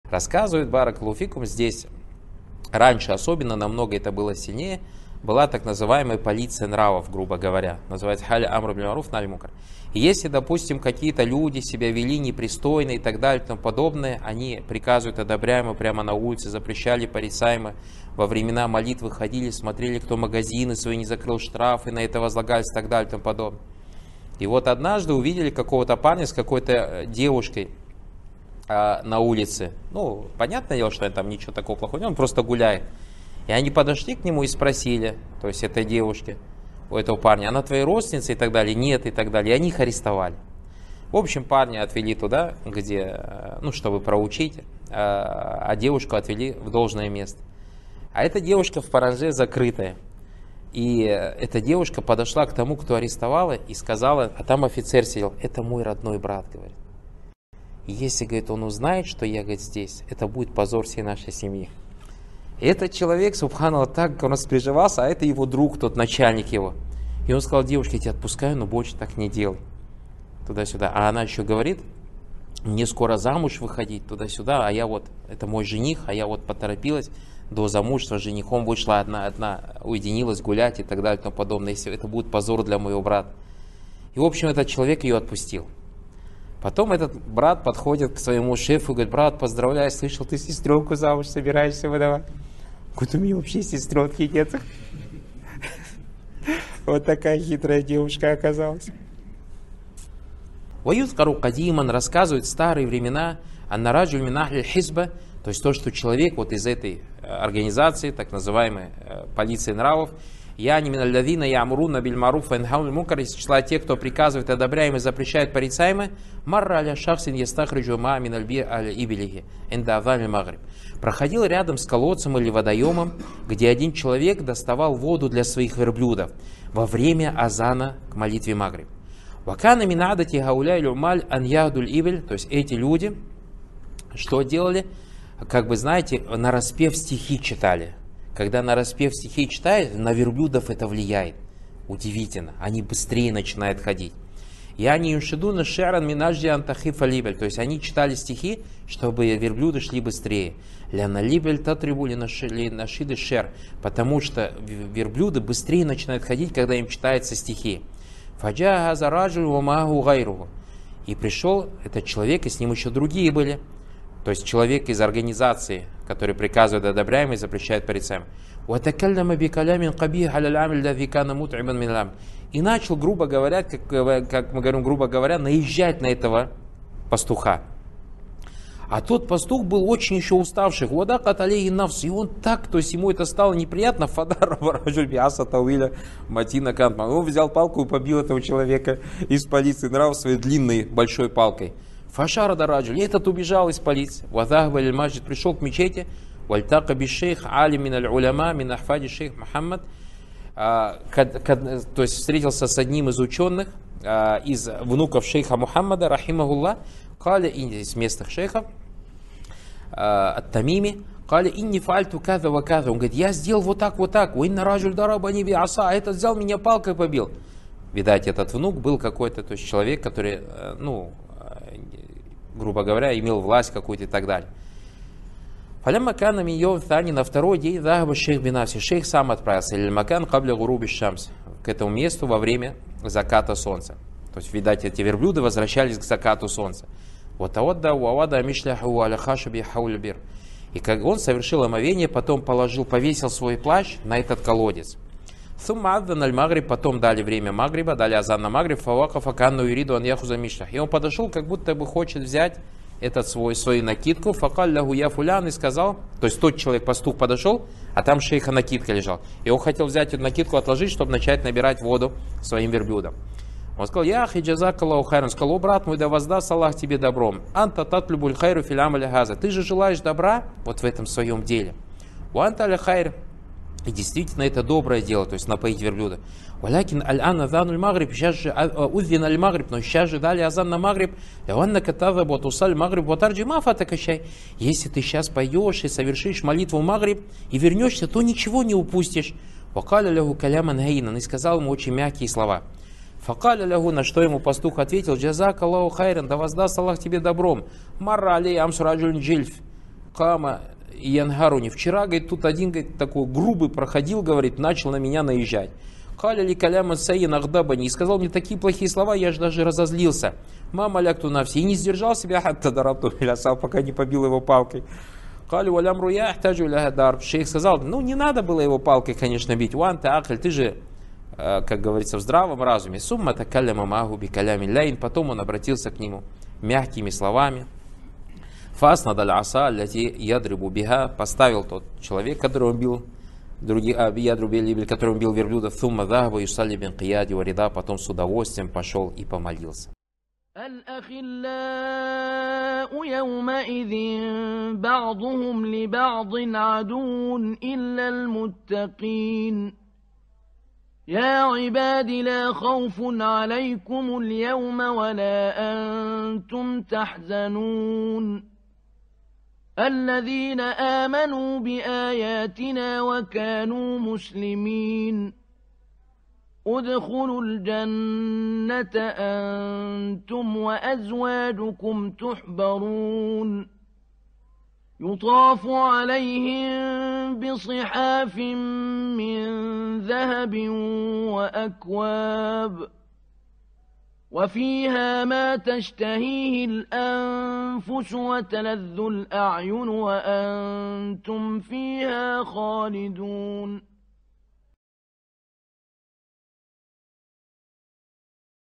Два случая из практики полиции нравов. Ринат Абу Мухаммад. Пользы из Садов праведных
🚩 ВКонтакте 🚩 👉 Смотрите полностью 23.1 При каких условиях дозволено порицать грешника? Вступление к главе 23 | Сады праведных.

🎧 Хадис 34. Устранение порицаемого является исламской обязанностью

✅ Наши сайты:
1. Официальный сайт «К Исламу»
2. Изучение таухида для начинающих
4. Аудио лекции для требующих знания